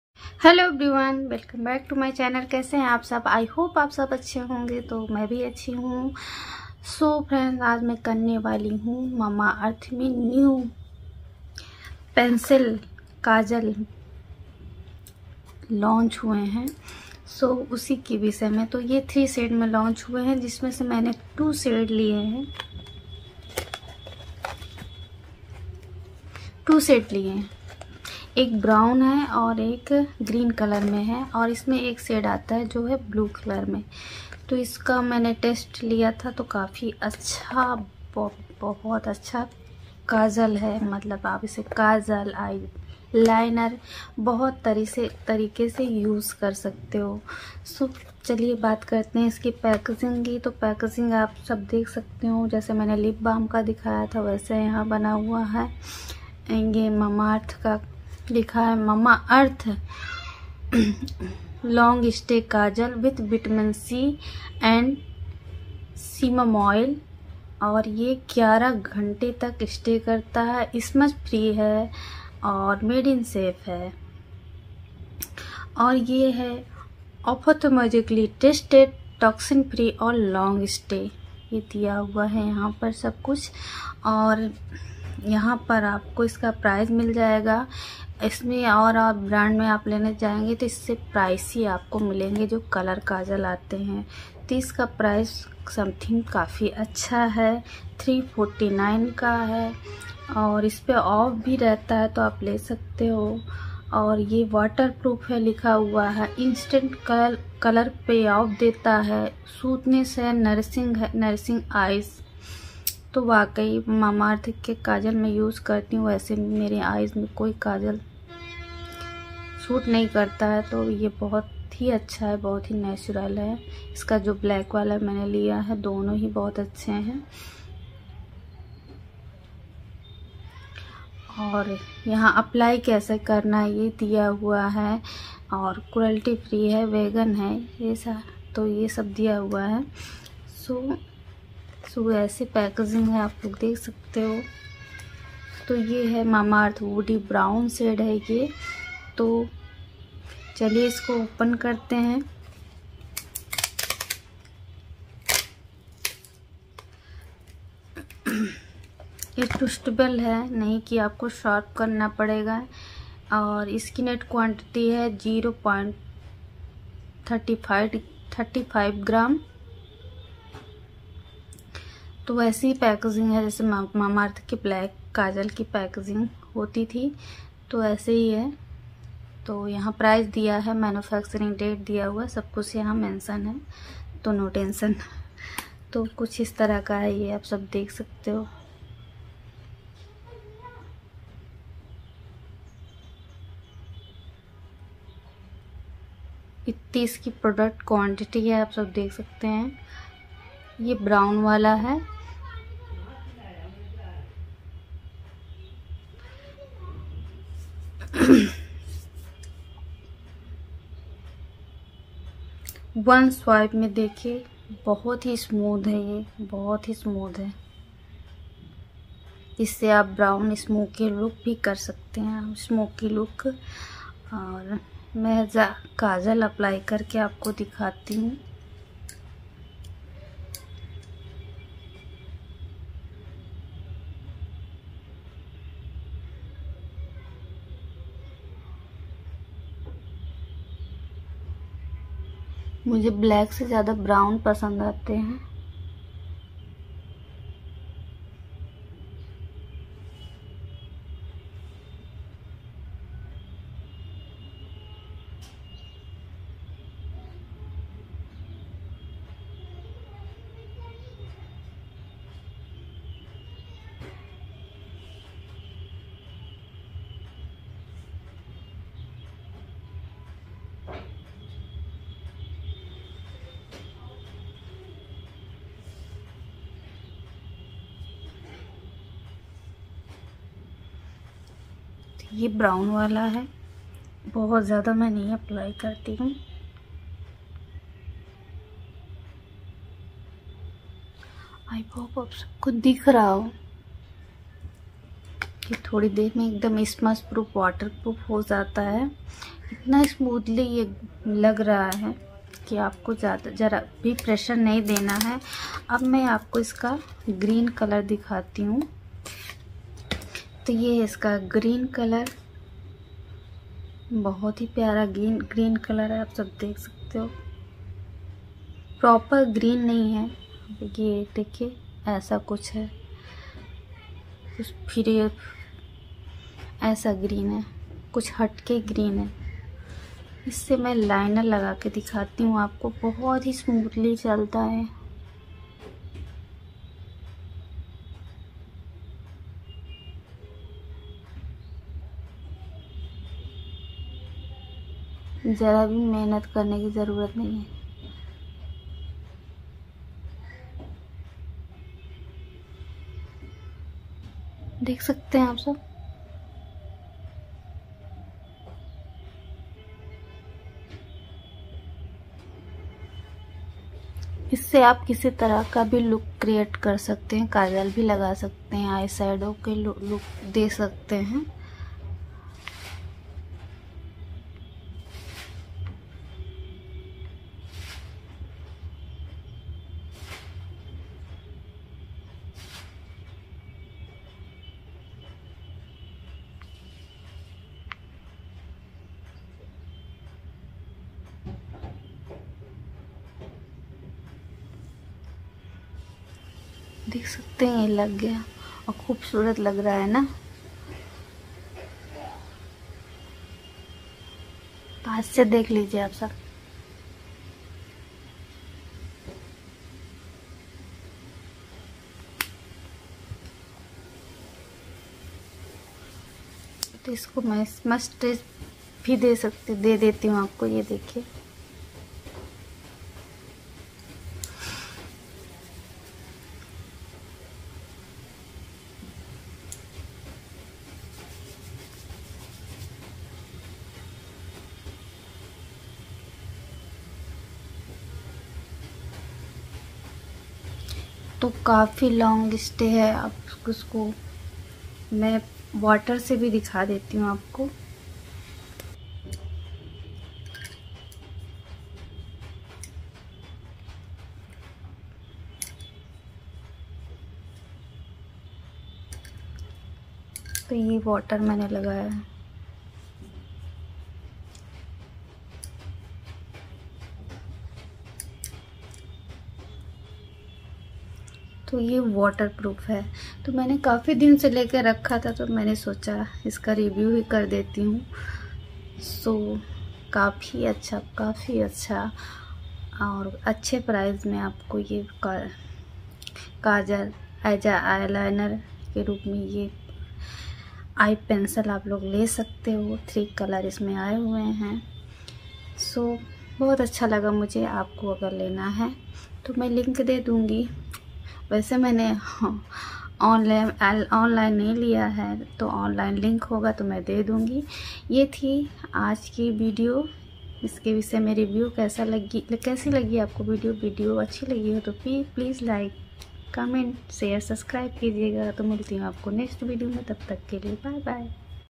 "New" Mamaearth Long Stay Colored Kajal For "11" Hour Long Stay | Review | 3 shade
instagram I'd

mamaearth long stay colored kajal

Mamaearth Lipstick